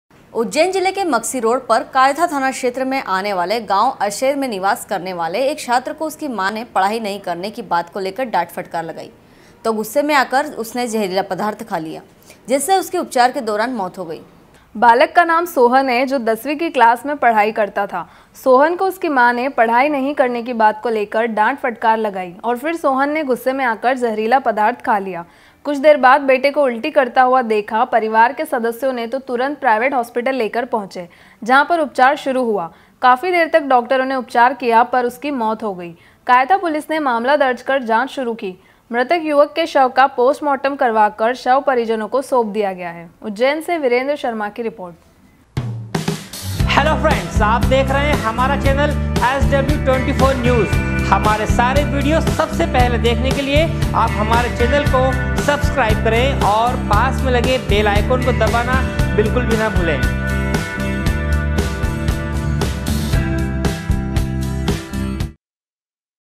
ujjain- उज्जैन जिले के मक्सी रोड पर कायथा थाना क्षेत्र में आने वाले गांव अशेर में निवास
उज्जैन जिले के मक्सी रोड पर कायथा थाना क्षेत्र में आने वाले गांव अशेर में निवास करने वाले एक छात्र को उसकी मां ने पढ़ाई नहीं करने की बात को लेकर डांट फटकार लगाई तो गुस्से में आकर जहरीला पदार्थ खा लिया जिससे उसकी उपचार के दौरान मौत हो गई। उज्जैन से विरेन्द्र शर्मा की रिर्पोट।